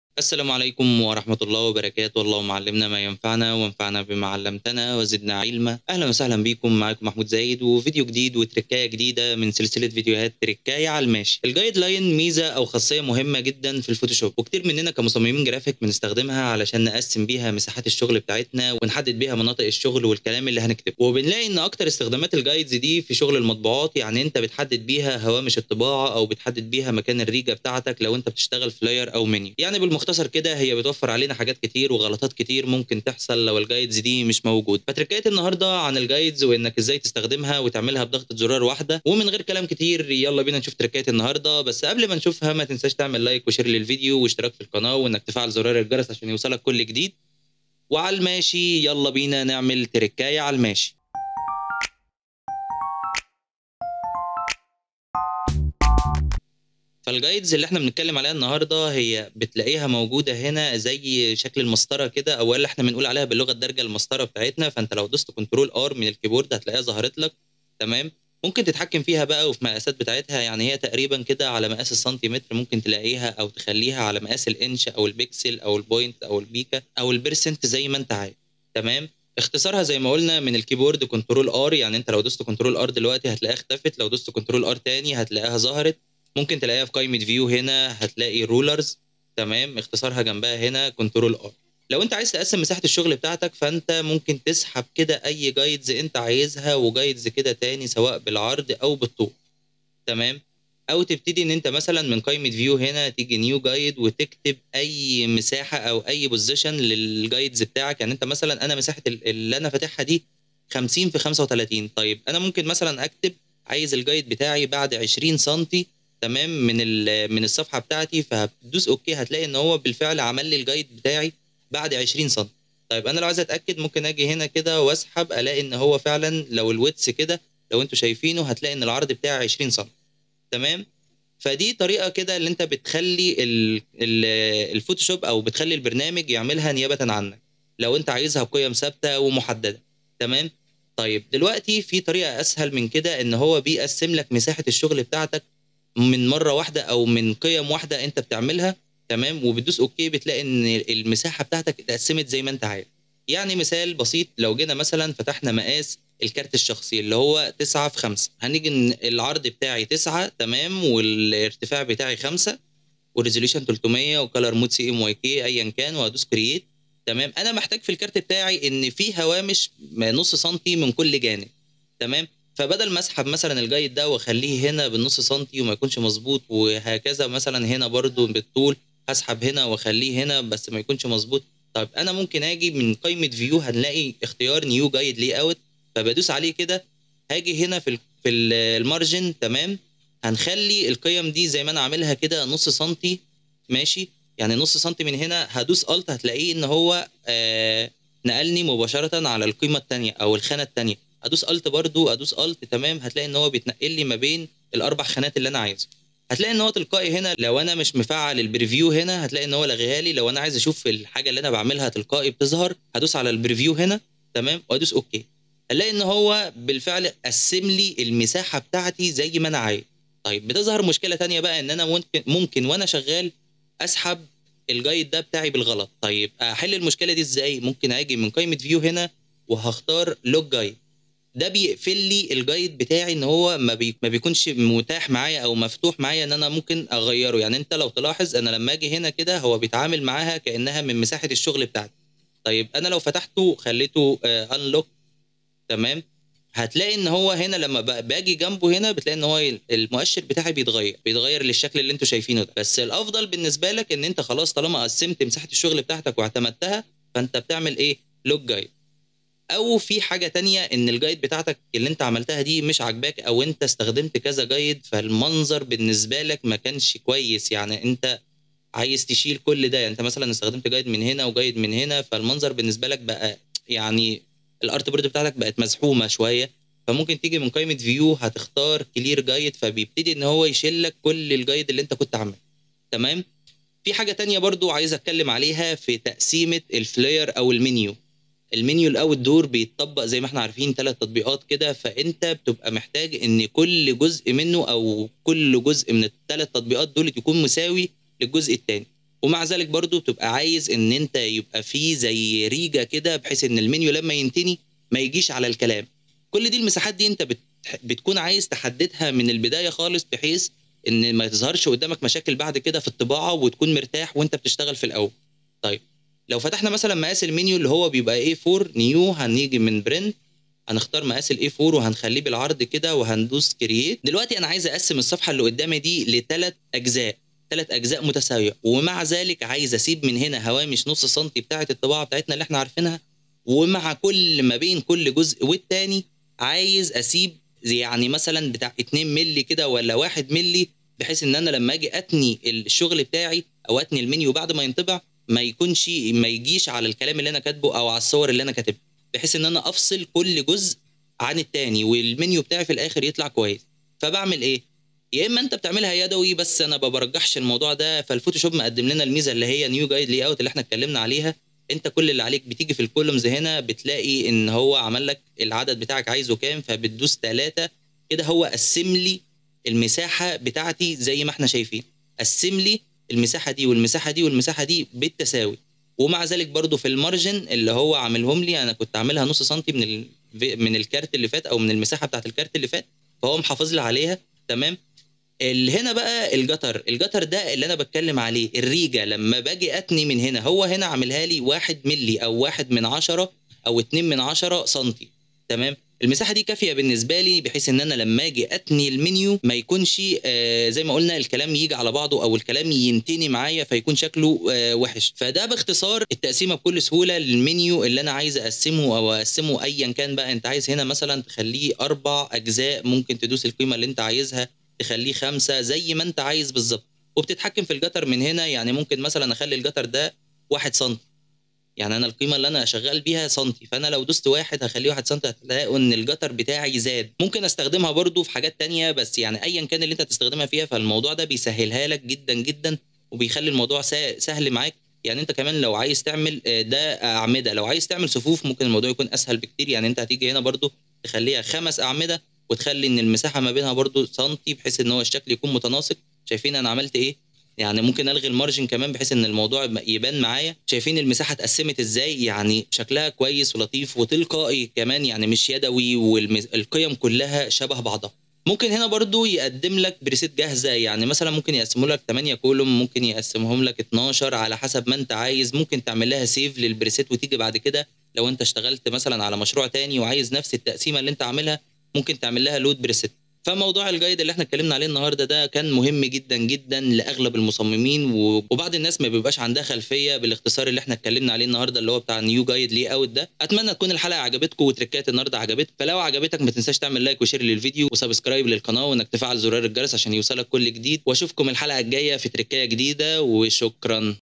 إزاي تقسم شغلك بضغطه زرار واحده
في الفيديو ده وفي تركايه النهارده هنتعلم نقسم مساحه الشغل بتاعتنا علي برنامج الفوتوشوب بضغطه زرار واحده .